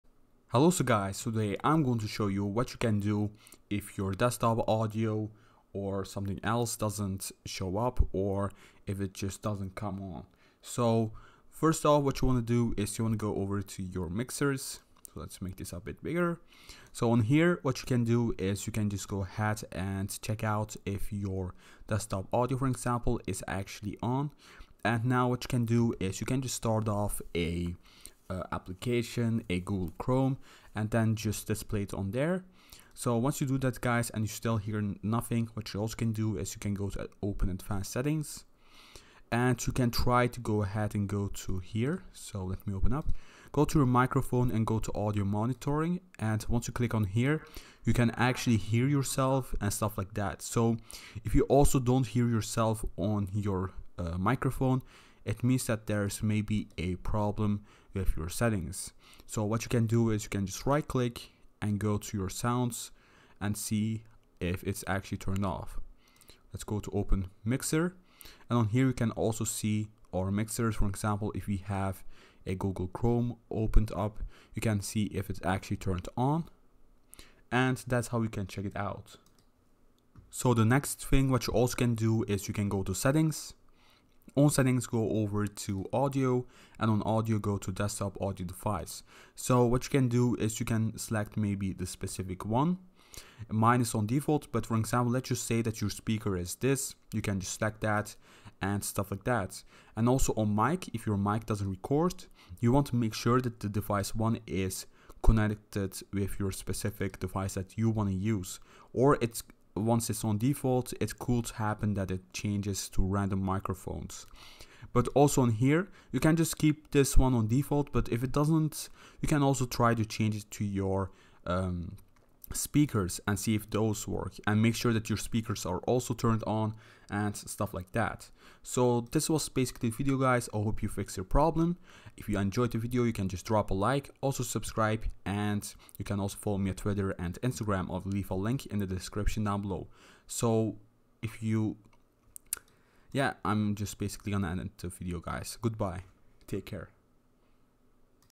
Streamlabs OBS How to FIX No Desktop Audio!
This video shows how to show no desktop audio and sound on streamlabs obs